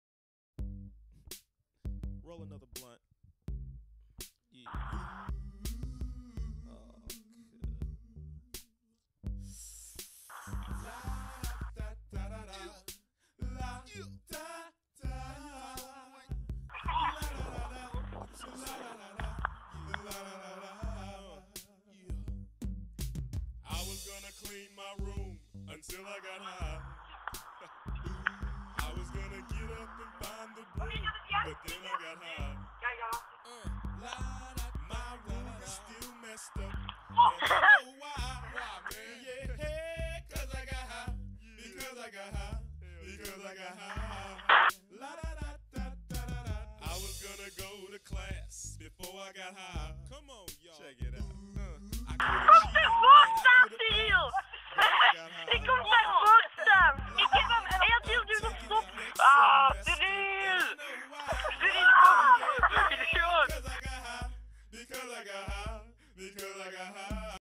Mislukte Vlinderbom...
we tried to make a deodoranthingy explode...